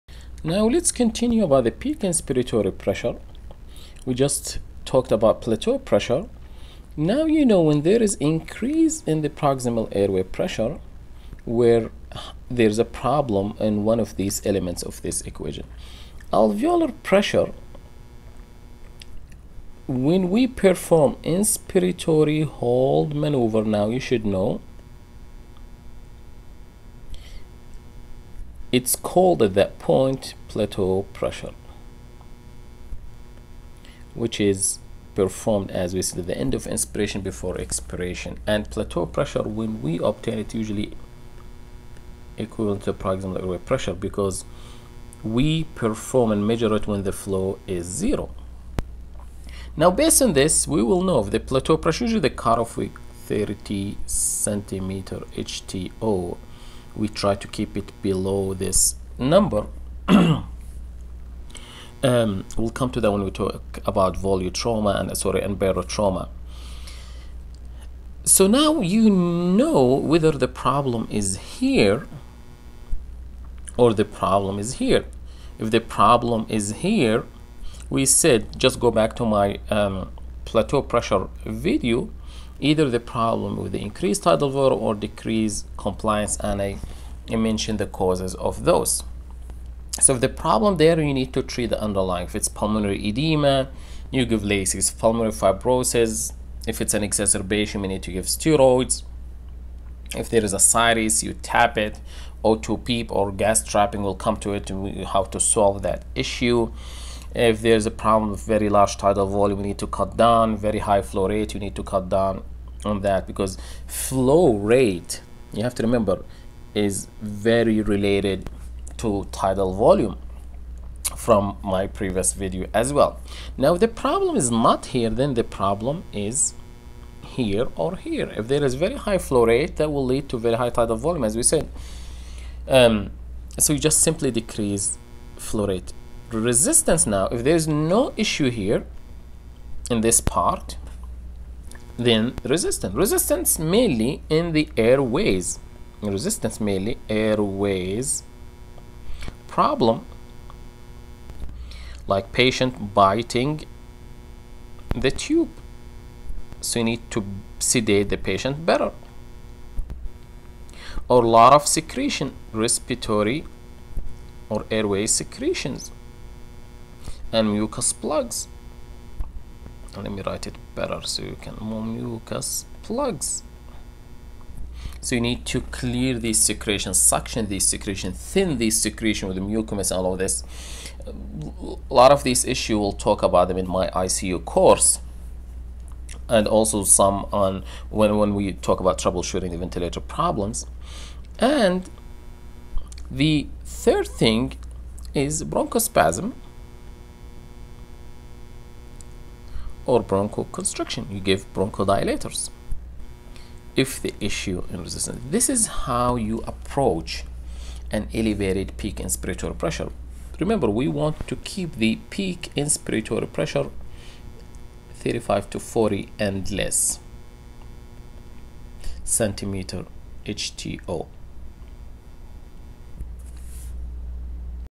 Mechanical ventilation- 48: Peak inspiratory pressure (part 2)
We explain how to troubleshoot a high peak inspiratory pressure, is it a  result of increased airway resistance or decreased compliance ( high plateau pressure )?